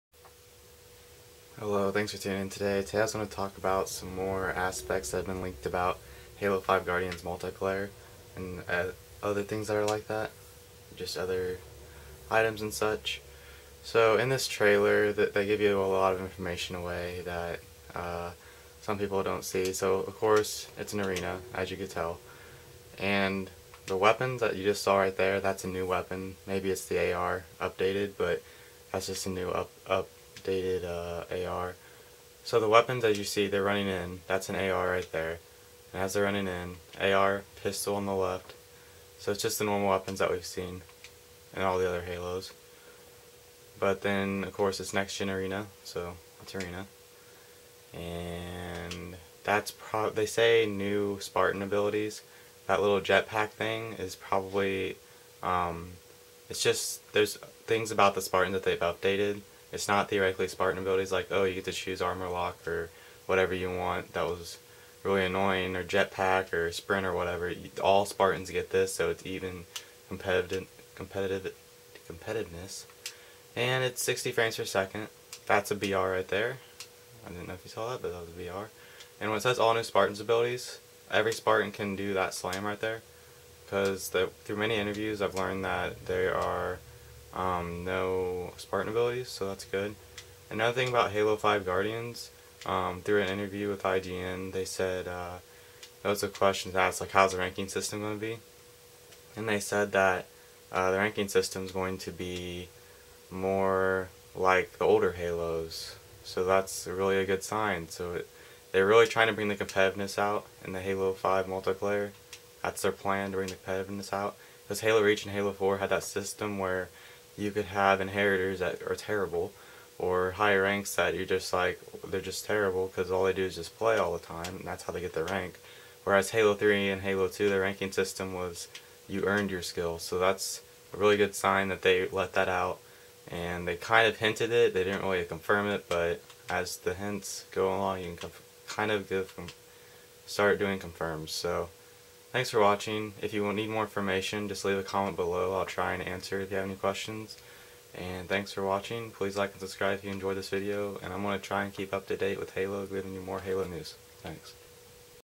Halo 5 Guardians - New Multiplayer Info, Ranking System, No Spartan Abilities, Weapons and Much More
In this video I will explain hinted and confirmed information about Halo 5 Guardians Multiplayer

I will also explain about what to expect in the Halo 5 Guardians Beta

A lot of these aspects may be similar in the Halo: Master Chief Collection Multiplayer (For example, Ranking System)